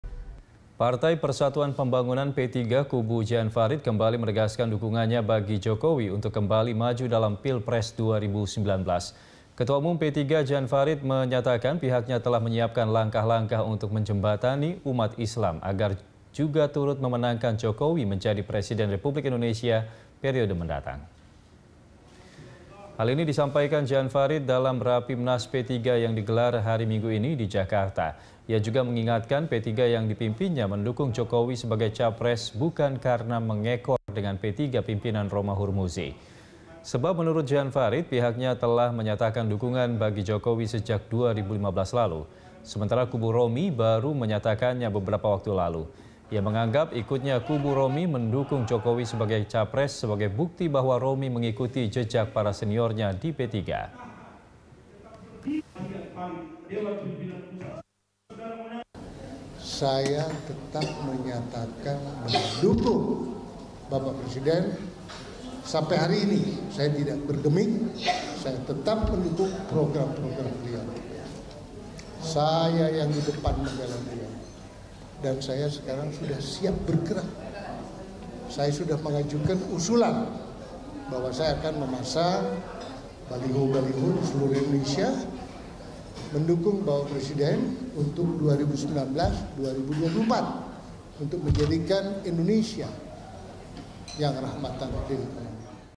PPP Kubu Djan Faridz Tegaskan Tetap Dukung Jokowi dalam Pilpres 2019
PPP Kubu Djan Faridz menegaskan tetap mendukung Joko Widodo dalam Pemilihan Presiden 2019.

Facebook.com/BeritaSatuTV
Youtube.com/BeritaSatu